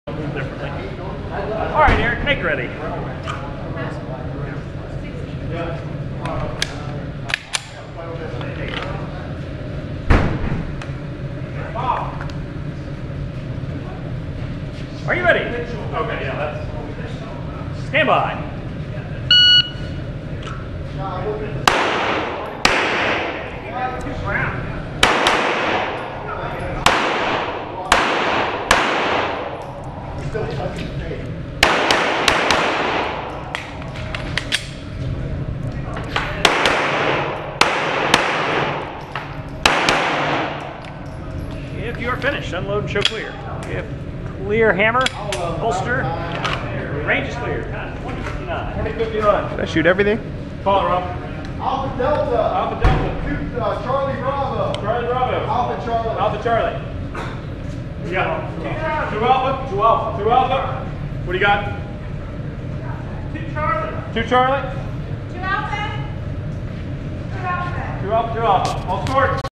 GPSL Stage 1 - Bob and Weave - May 3, 2016
Stage 1 at the May 3, 2016 Gwinnett Practical Shooting League match, which is held at Range, Guns & Safes in Norcross.

An interesting course shaped like a T. I happened to shoot after one of the GMs and discovered that engaging the side target right off the buzzer meant that you could basically eliminate shooting either side target from the middle and skip right to the end. It pays to watch your "elders"!

Glock 34
Dawson sights